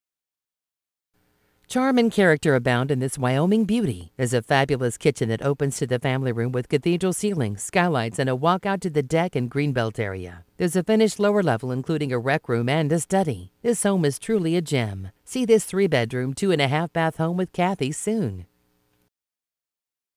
Home for sale in Wyoming, OH | $249,900
Charm &amp; character abound in this Wyoming beauty! Fab kitchen opens to fam room w/cath ceil, skylights &amp; walkout to deck &amp; greenbelt area! Fin LL incl rec rm &amp; stucy. Truly a gem!!